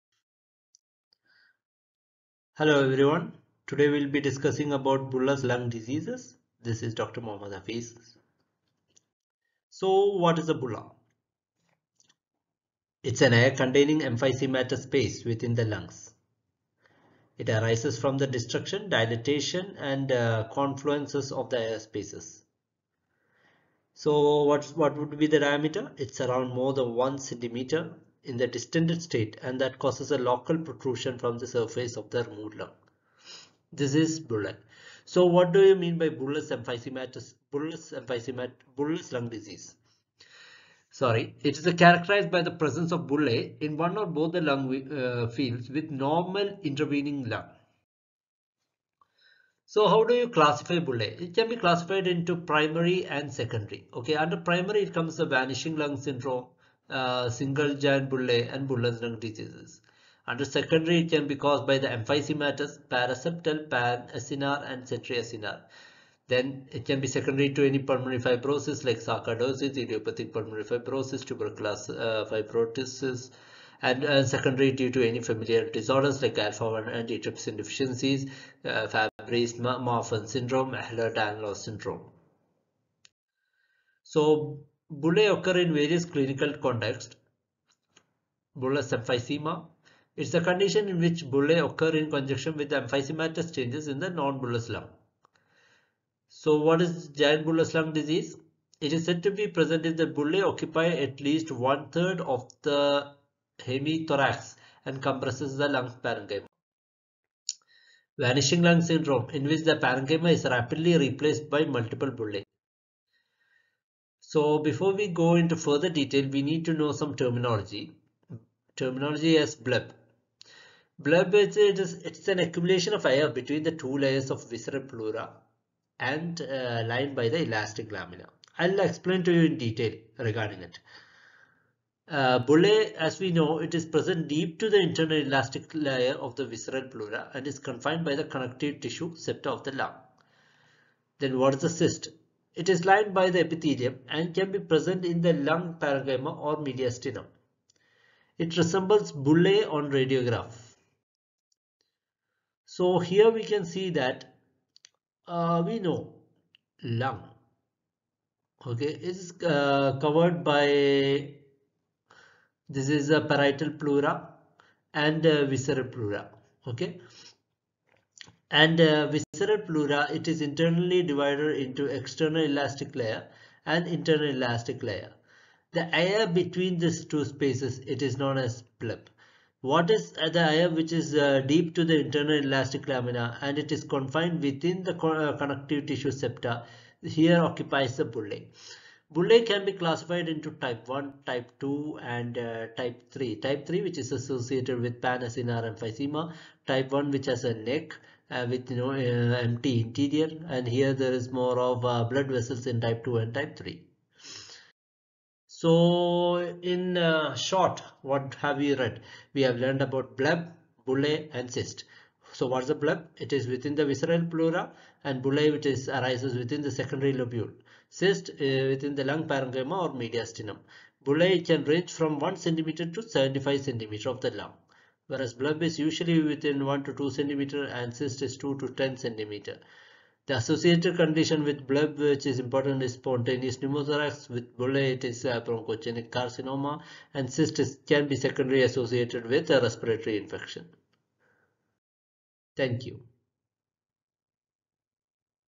Bullous Lung Disease- Part I
Classification of bullous lung disease
Terminiology
Differences between Bleb, Cyst and Bullae